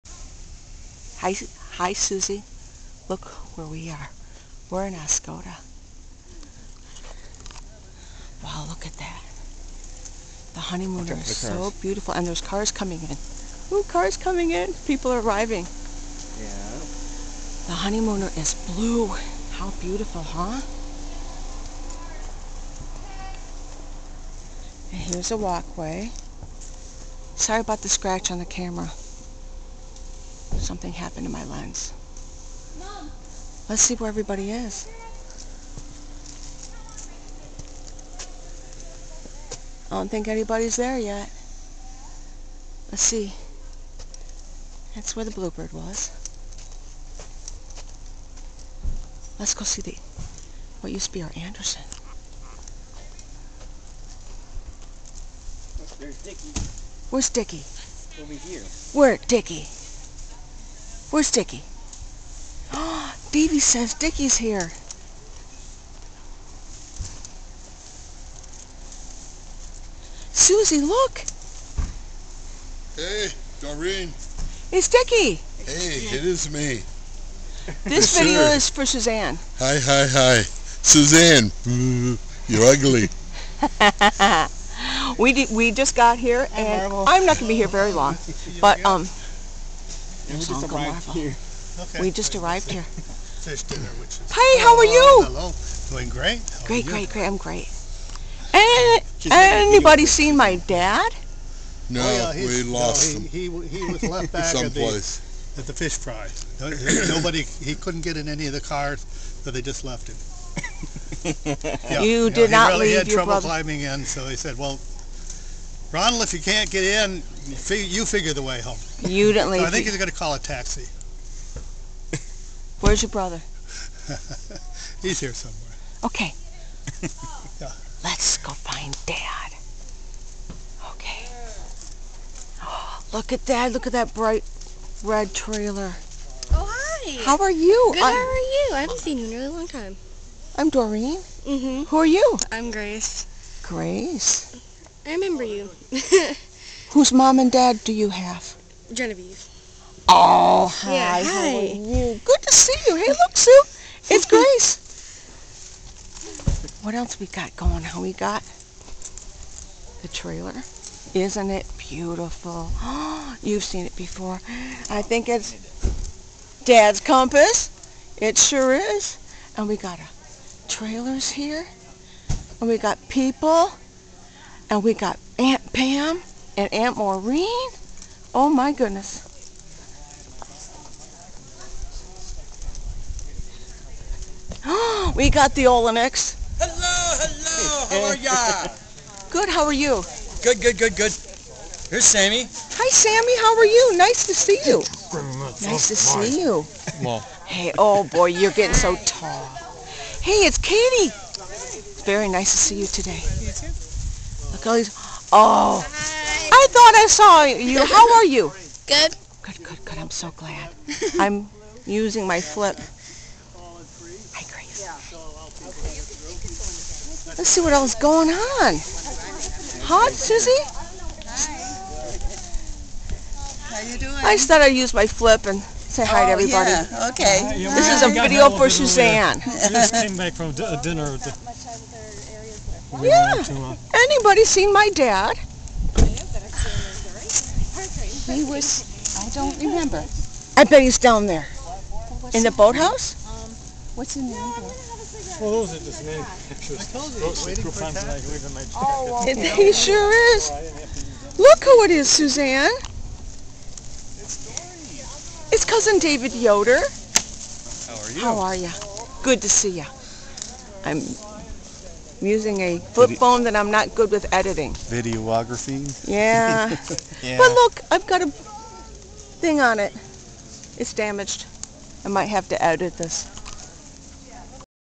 Oscoda: Where's Dad? Aug 2014
Arriving at Oscoda, MI, Aug 2014.